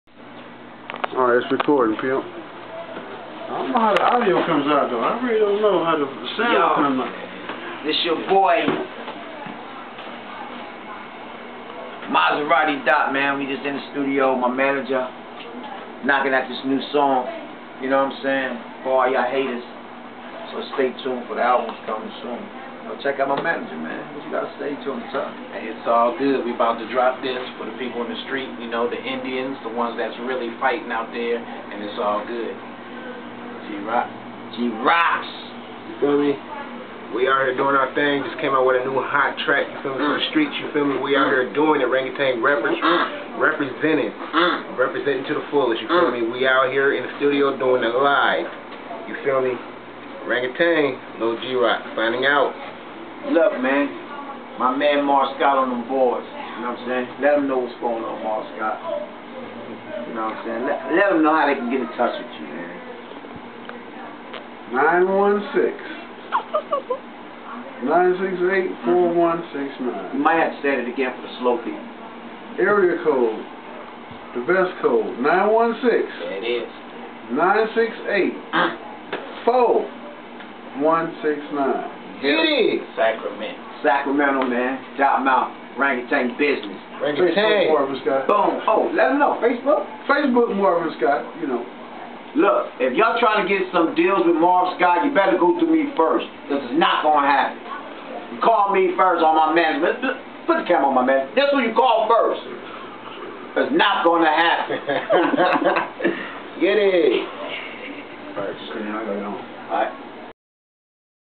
IN THE STUDIO WITH THE FAM!!!!
At the RANGUTAN HUNT WITH MY MGR AND LILGEEROCKS DJ PREZZ CAME OUT TO THE SPOT GOOD LOOKING OUT BIG HOMIE!NEW SONG COMING SOON HERE IS THE VIDEO HAVING SOME FUN TO NITE TO BOOK ANY RANGUTAN RECOTDS GO TO